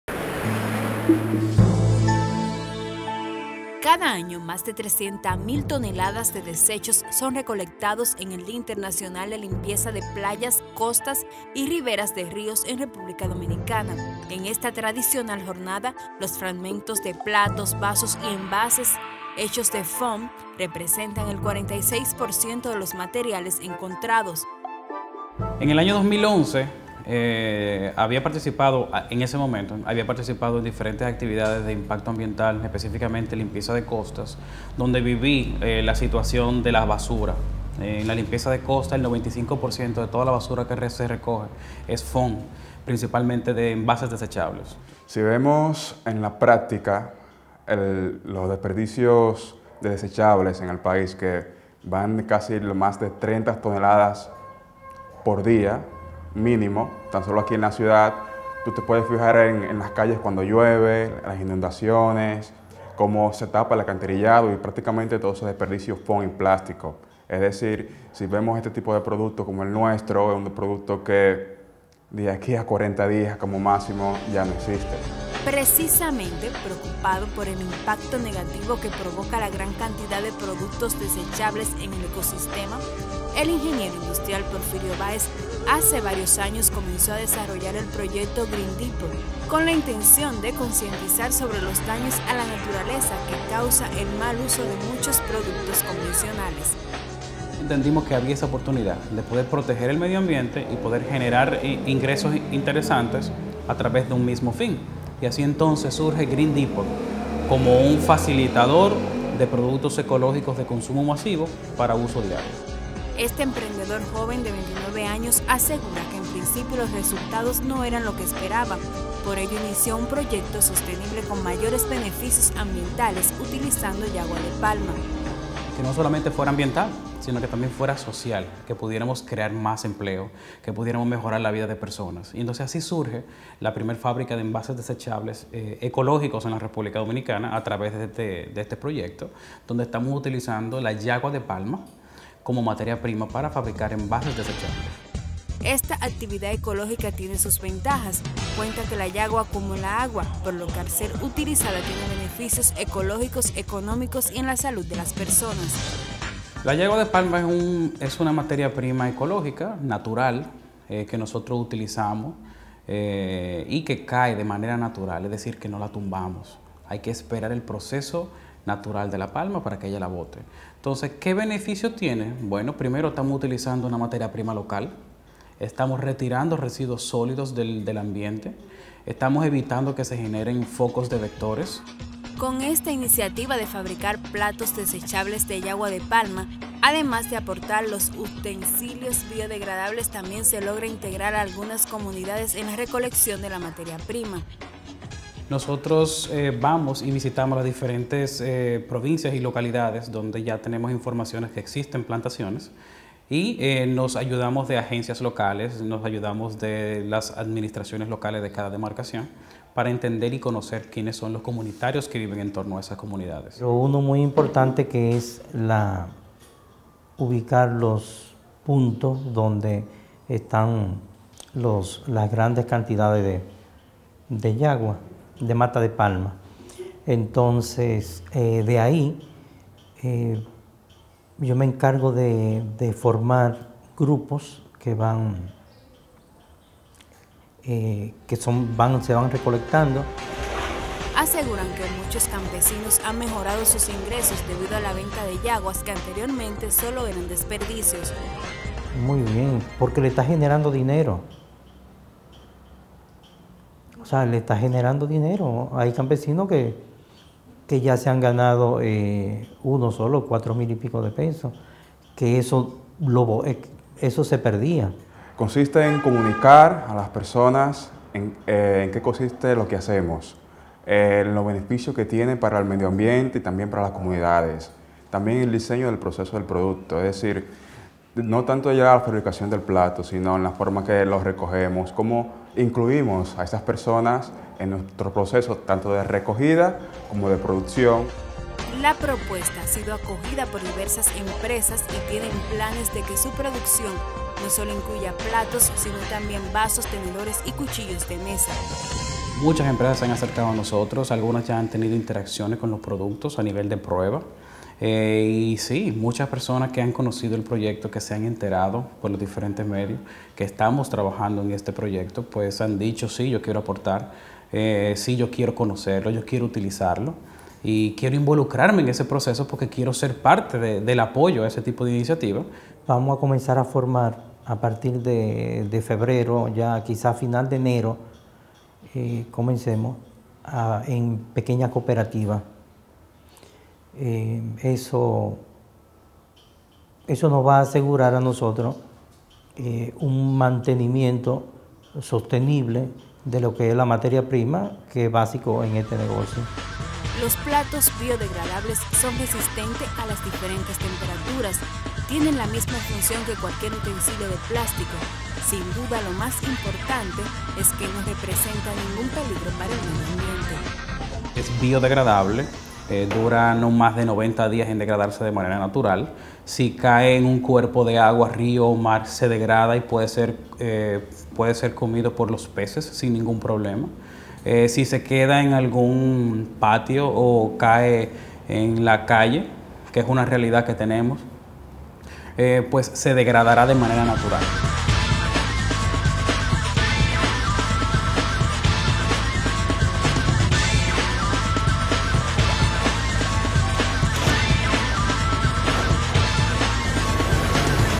Hce Platos de Yagua de Palma - Grandioso!!
la iniciativa de un joven con un alto sentido del respeto y la conservación medioambiental. Su nombre es Porfirio Báez y a sus 29 años ha logrado encontrar la forma de elaborar platos desechables utilizando yaguas de palma. Olinda Payano nos explica el proceso ideado por este brillante dominicano.